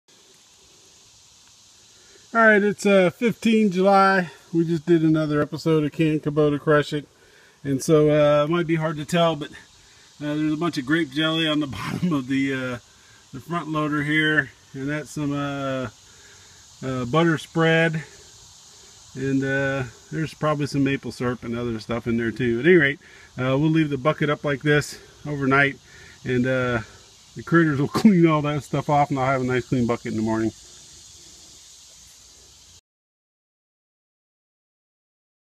Secret Trick on How to Clean the LA525 Front Loader Bucket on Your L3301 HST Kubota Diesel Tractor
The Secret Trick on How to Clean the LA525 Front Loader Bucket on Your L3301 HST Kubota Diesel Tractor.